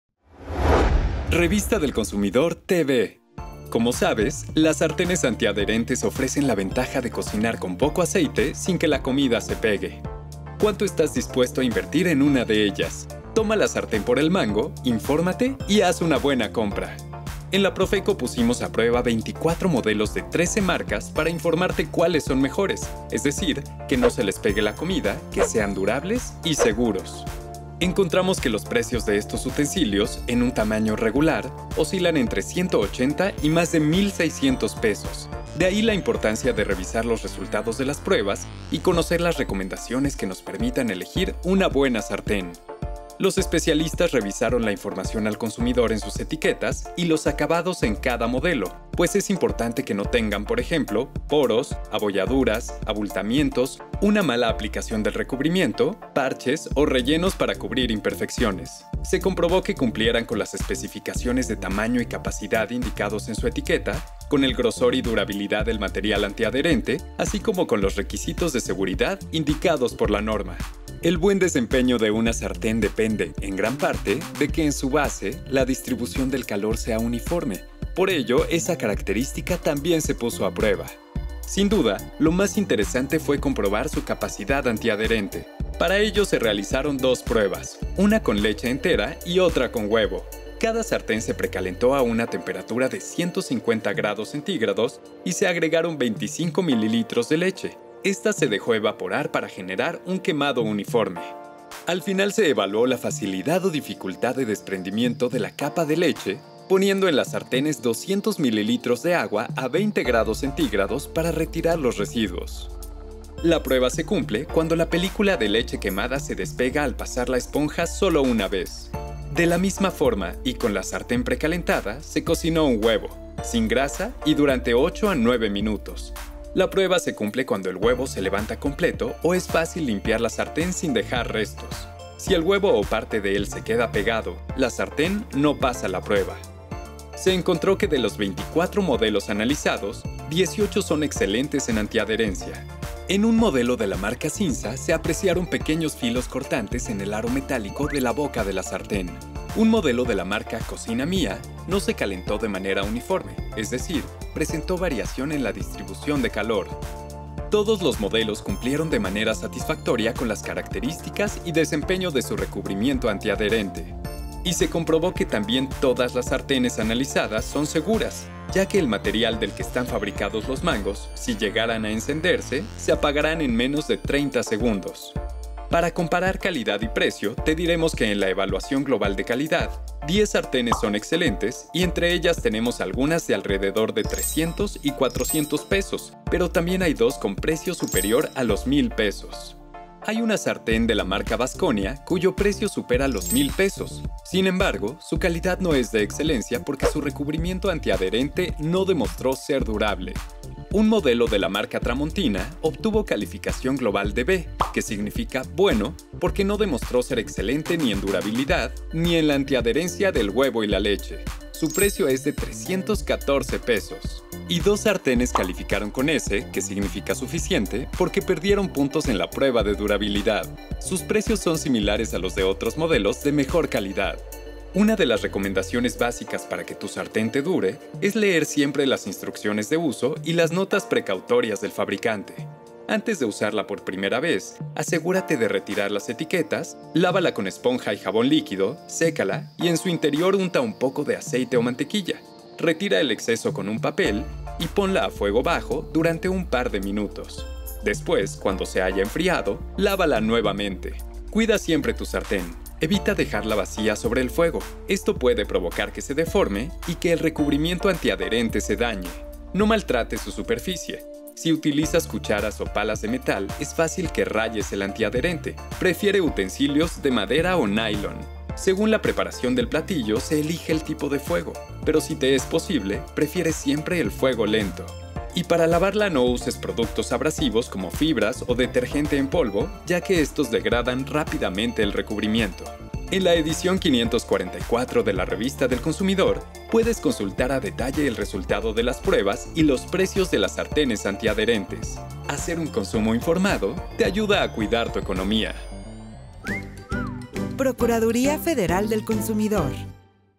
Sartenes antiadherentes | Estudio de Calidad | Profeco
Ya sea para preparar unos huevos a la mexicana o asar un bistec, elegir una buena sartén no tiene porqué ser difícil.

Las sartenes antiadherentes ofrecen cocinar con poco aceite sin que la comida se pegue, por eso, en nuestro EstudioDeCalidad pusimos a prueba este utensilio de cocina utilizado a diario en muchos hogares.

Conoce su calidad, elige y cocina informado.